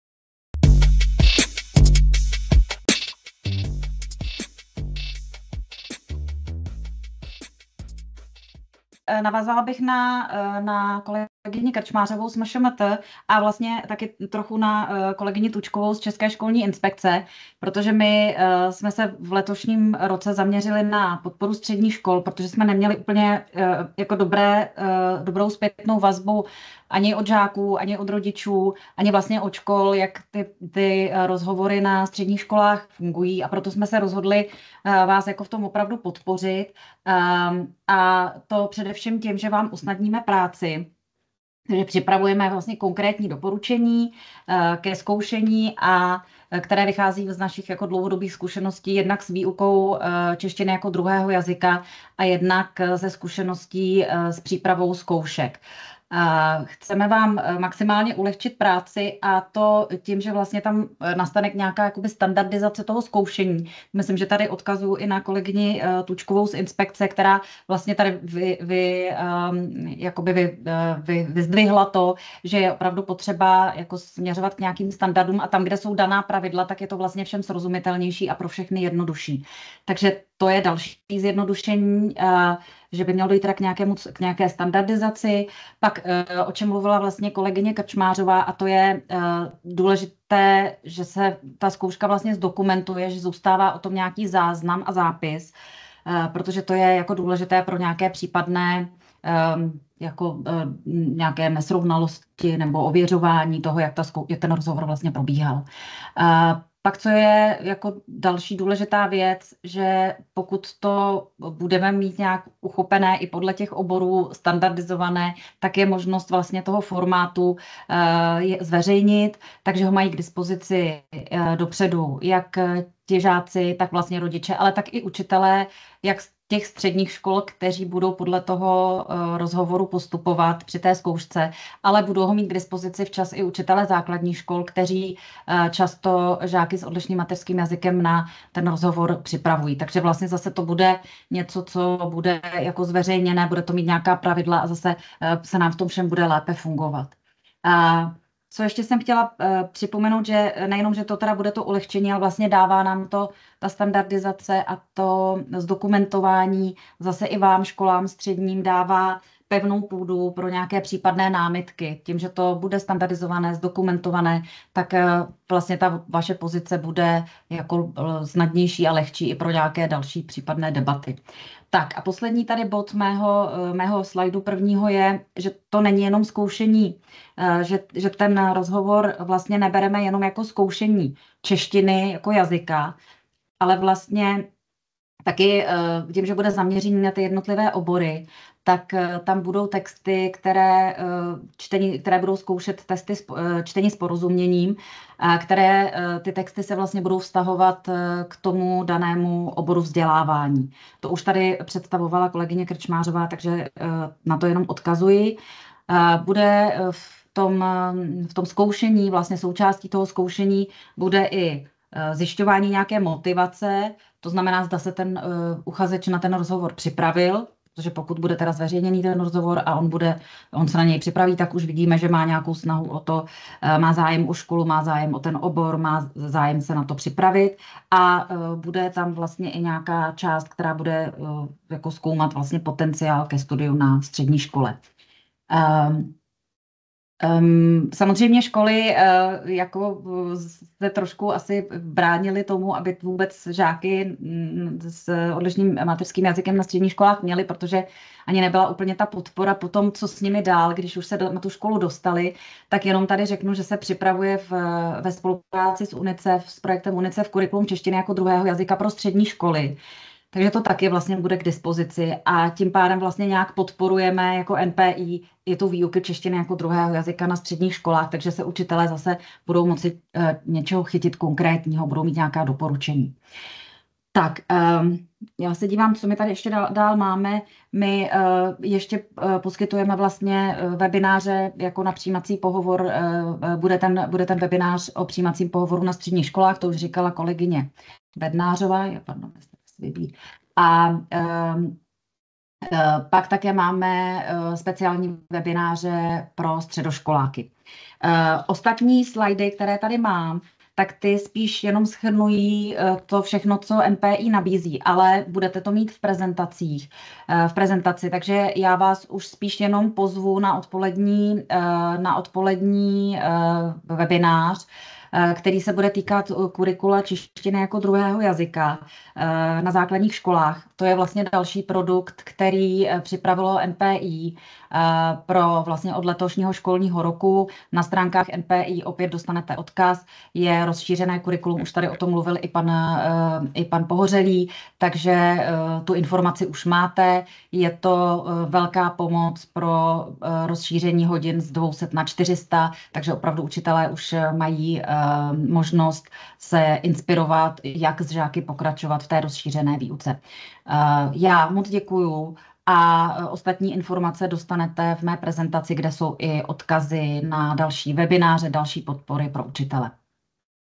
Konference Přijímáme, začleňujeme a vzděláváme DŽC 2024: Představení podpory NPI v oblasti češtin...
Podívejte se na záznam z 8. ročníku online konference Přijímáme, začleňujeme a vzděláváme děti/žáky cizince, která se uskutečnila 10. října 2024.

➡ Záznamy z dalších částí konference a prezentace ke stažení, najdete na stránce